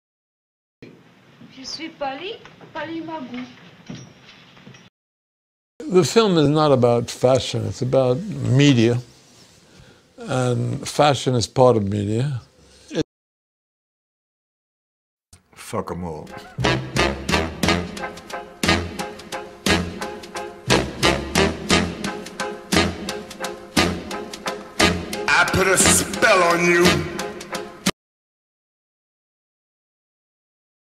Motion- The Films of William Klein
William Klein is widely known for his fashion photography for Vogue and also his photographic journals of many cities. This project aims to appeal to highly visual people, who may be aware of the photography work of William Klein but less or completely unaware of his film-making.